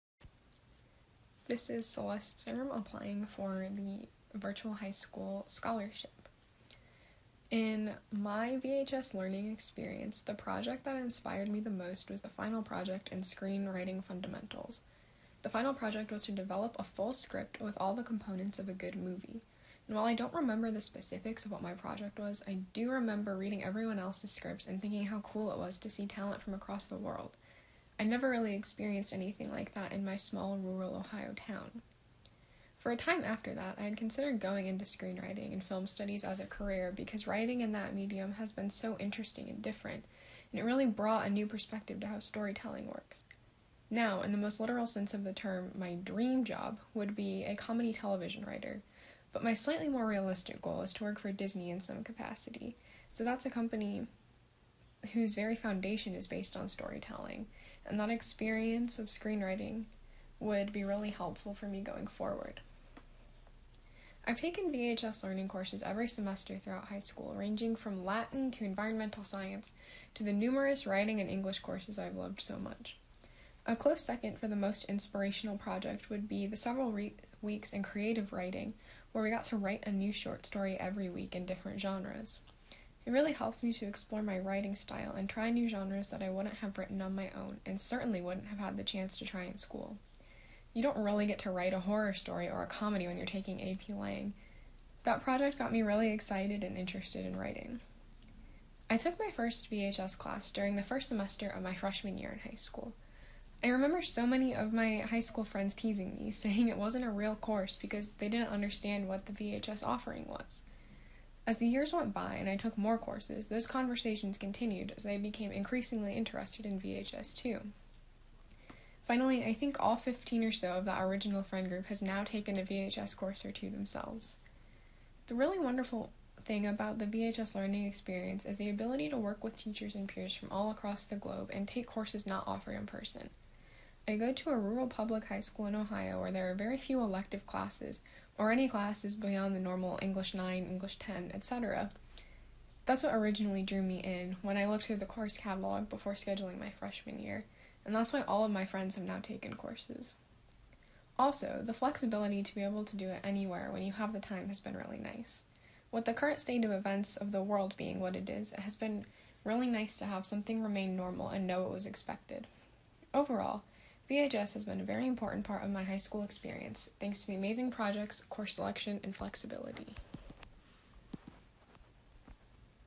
2020 VHS Learning Scholarship Second Runner-Up - Celeste Z.
We receive lots of feedback that students love the projects and assignments in their VHS Learning courses. From the group discussions to the hands-on labs, students have found inspiration everywhere. So this year, we ask students to tell us:

“In your VHS Learning course, what project or activity inspired you most? Explain why."